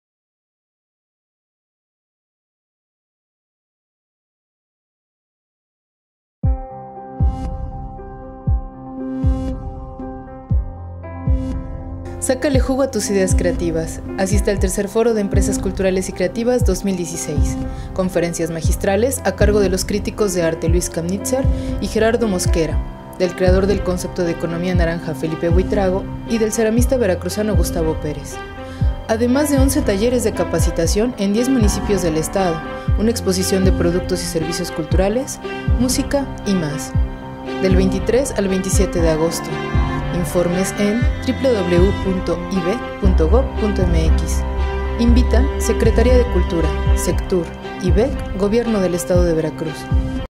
Tercer Foro de Empresas Culturales 2016
Del 23 al 27 de agosto asiste al Tercer Foro de Empresas Culturales y Creativas del IVEC. Habrá conferencias magistrales, talleres, exhibiciones y presentaciones artísticas. ¡Sácale jugo a tus ideas creativas!